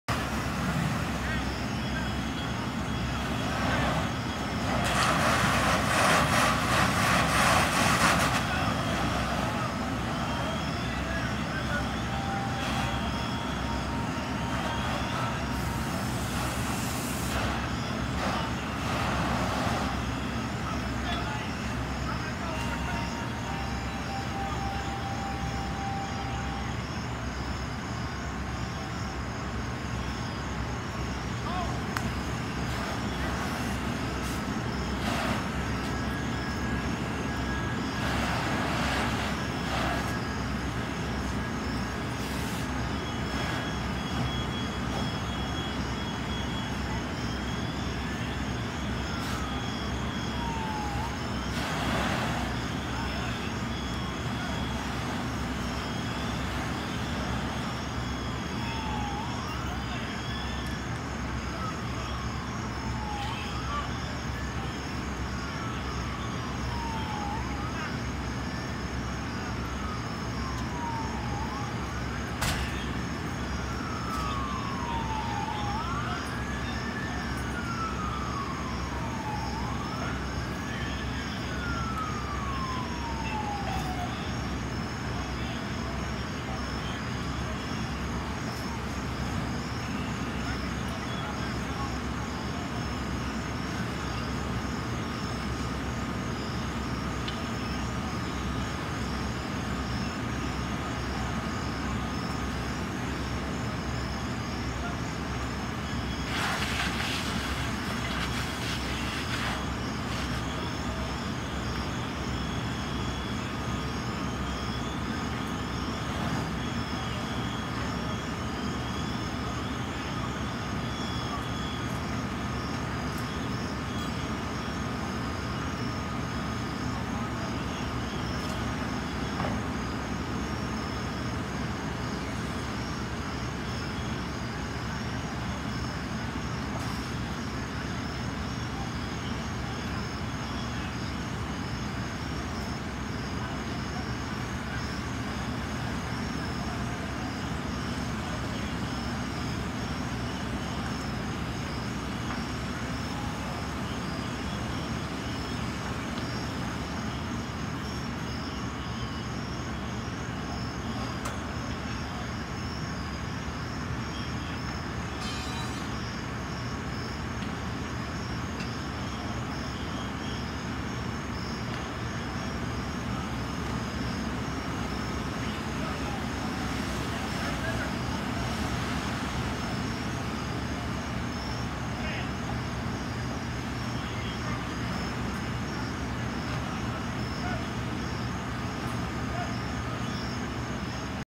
08/29/18 Working Commercial Structure Fire, Statesville / Iredell County NC
Large Commercial Structure Fire

Monticello, Stony Point, Trinity, Statesville E-2, Iredell Rescue, ICEMS - Commercial Structure Fire - 282 Scott’s Creek Rd @ Benfield Sanitation - Reported Flames Visible

Update - Chief 800 Reporting Flames Visible From Hwy 115.  ICSO On Scene Confirming Working Fire.

Update - Scotts Creek Rd Closed In The Area Due To Supply Line.  Command Requesting Energy United To The Scene.

Update - Command Request Second Alarm.  Adding West Iredell.

Update - Command Requesting Ladder Truck From West Iredell.  Also Requesting Five Additional Tankers.

Update - Second Water Point In Front Of Food Lion Hwy 115

Update - Adding Large Tankers From Cool Springs, Ebenezer & Central

Update - Command Requesting Central & North Sector General Alarm For Manpower.  That Adds Wilkes-Iredell, Sheffield-Callahan, Union Grove, Harmony, County Line & Lone Hickory.  Five Counties.

SVL Free News